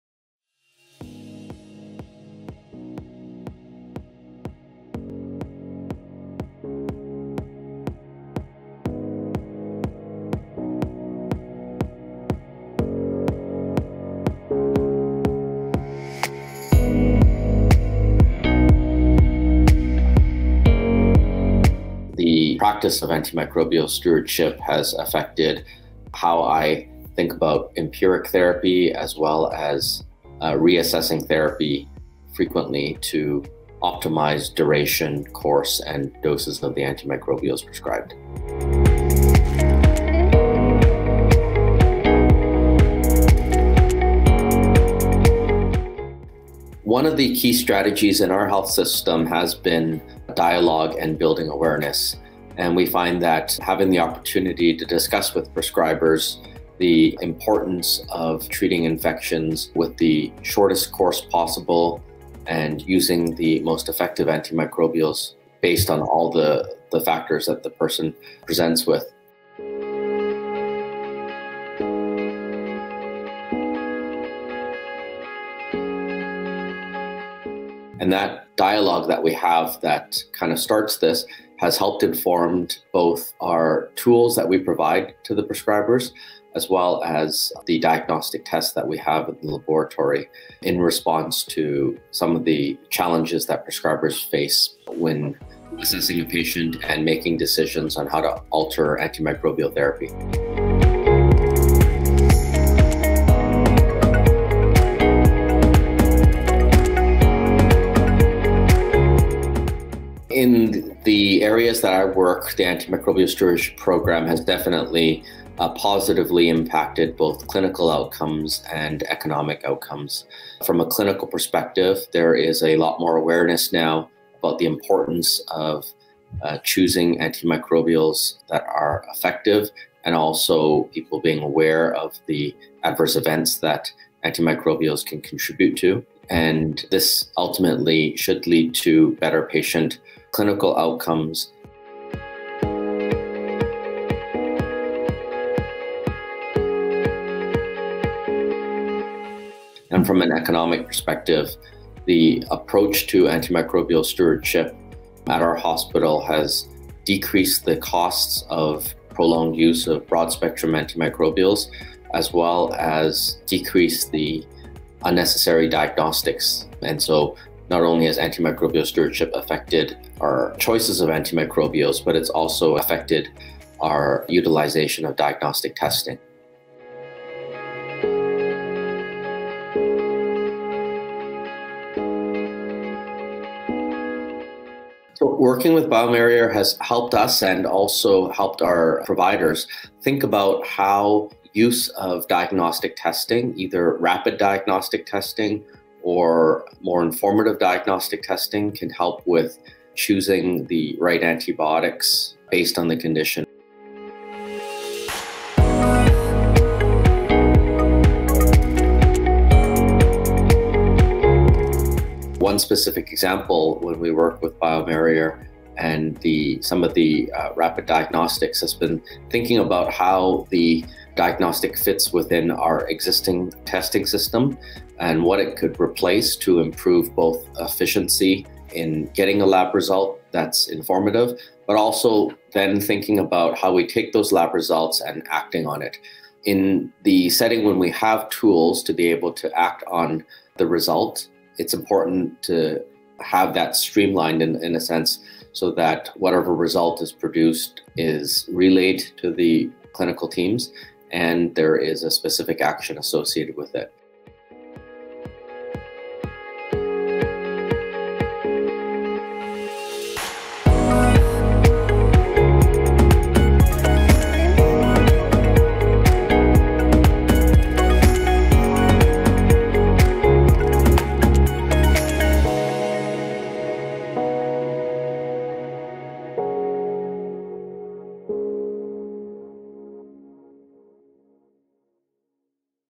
What Does a Successful Stewardship Program Look Like? Interview with Dr. Leung
Dr. Victor Leung shares how investing in antimicrobial stewardship strengthens his hospital’s ability to address antimicrobial resistance and manage infectious diseases.